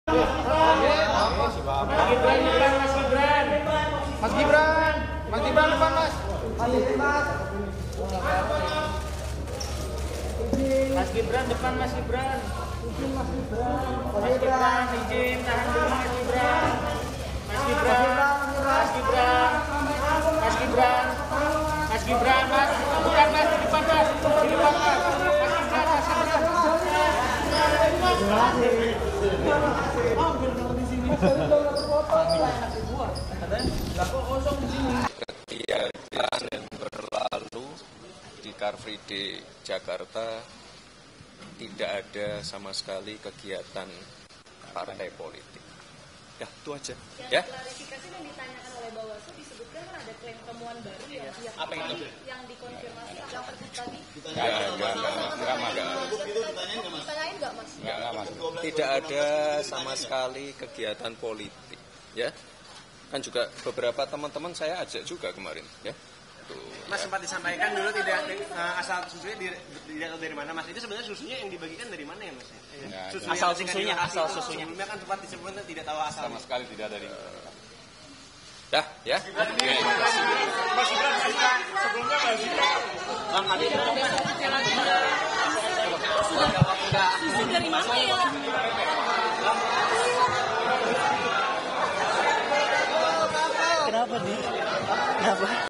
Usai Dipanggil Bawaslu Jakpus Terkait Bagi Bagi Susu di CFD, ini Keterangan Cawapres Gibran
JAKARTA, KOMPAS.TV – Cawapres Nomor Urut 2, Gibran Rakabuming memberikan keterangan pers usai dipanggil oleh Bawaslu Jakarta Pusat pada Rabu, (3/1/2024).

Pemanggilan Gibran tersebut diketahui terkait dengan kegiatan bagi-bagi susu di Car Free Day (CFD) Jakarta.

“Di car free day Jakarta tidak ada sama sekali kegiatan partai politik,” ungkap Cawapres Nomor Urut 2, Gibran Rakabuming.

Aktifkan juga lonceng supaya kamu dapat notifikasi video terbaru dari Kompas Sorong.